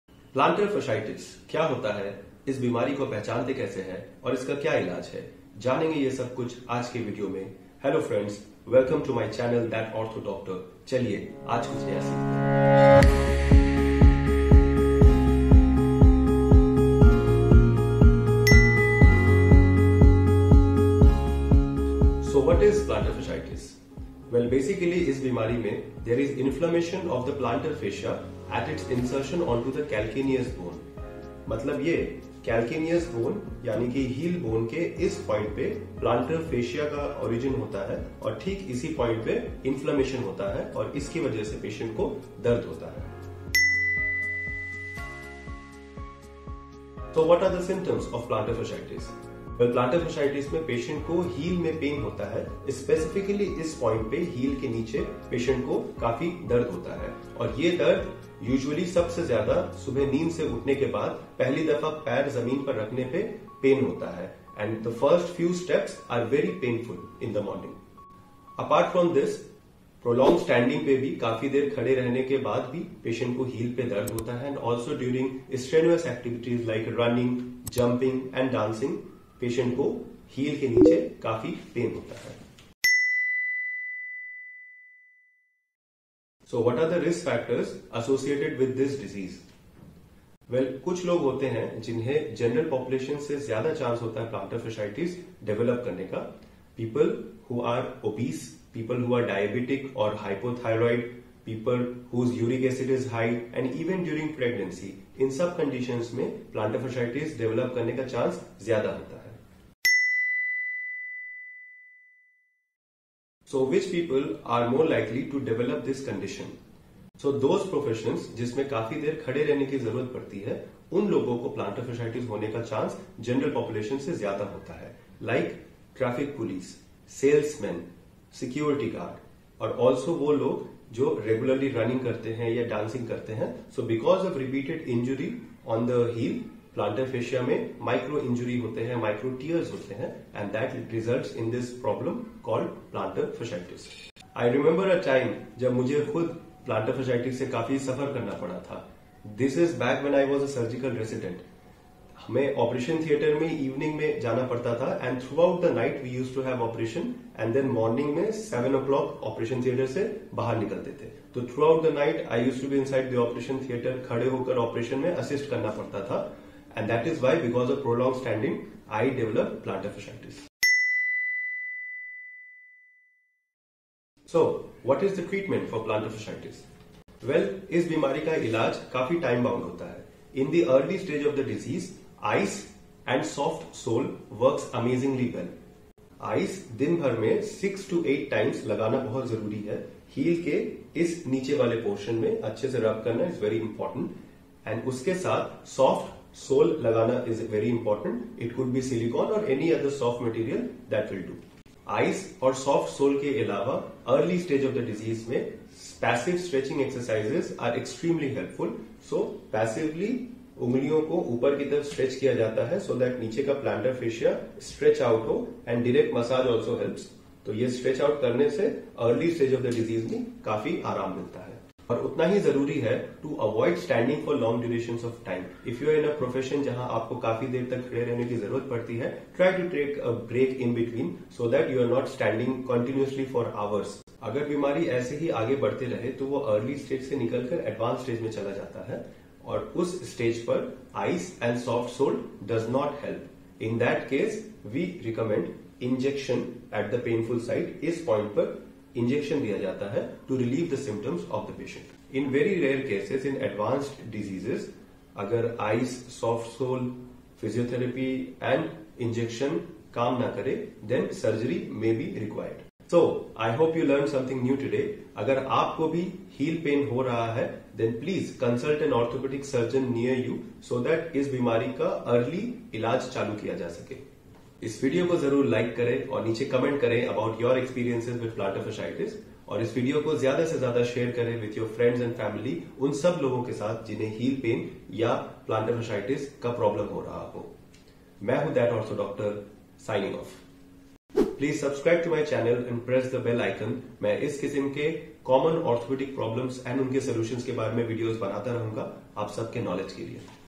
Heel pain / Plantar fasciitis - What are its causes and treatment?
Do you suffer from heel pain?
Does it pain to take the first few steps in the morning?

You could be suffering from Plantar Fasciitis!
Watch this video to learn all about this condition - what it is, what are its symptoms, what are its causes and risk factors, and how to cure it.

Chapters:
0:00 - Intro
0:18 - What is Plantar Fasciitis?
0:44 - What are its symptoms?
1:23 - What are its risk factors?
1:50 - Which people/ profession develop this condition?
2:50 - What is its treatment?
4:22 - Outro

Presented by:
Dr. Sharjeel Khan
MS (Ortho), DNB (Ortho), MBBS
Fellowship in Spine Surgery